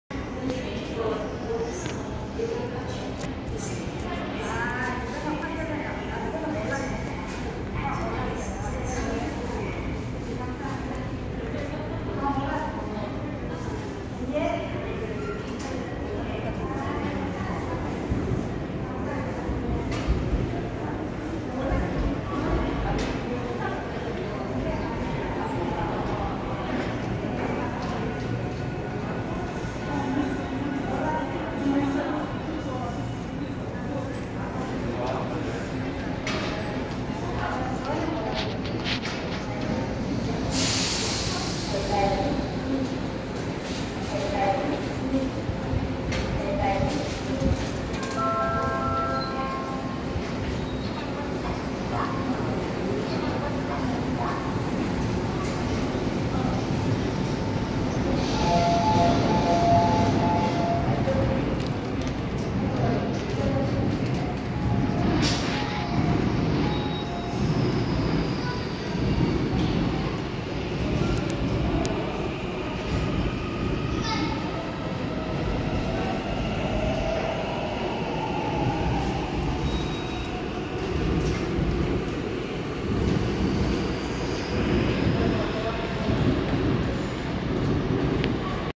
(20210410 105239)서울 지하철 7호선 온수행743편성 태릉도착,출발
다음 열차는 3차분!!